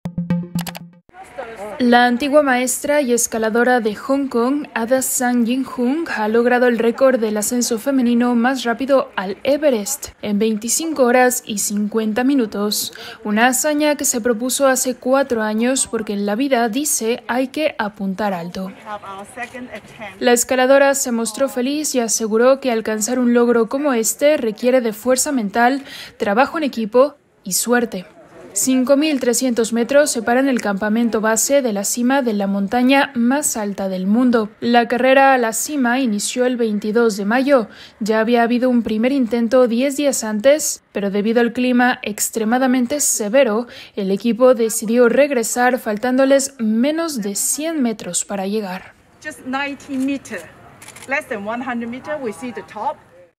El ascenso femenino más rápido del Everest: En la vida "hay que apuntar alto"
La antigua maestra y escaladora de Hong Kong Ada Tsang Yin-hung ha logrado el récord del ascenso femenino más rápido al Everest en 25 horas y 50 minutos, una hazaña que se propuso hace cuatro años porque en la vida "hay que apuntar alto".

¡Visítenos!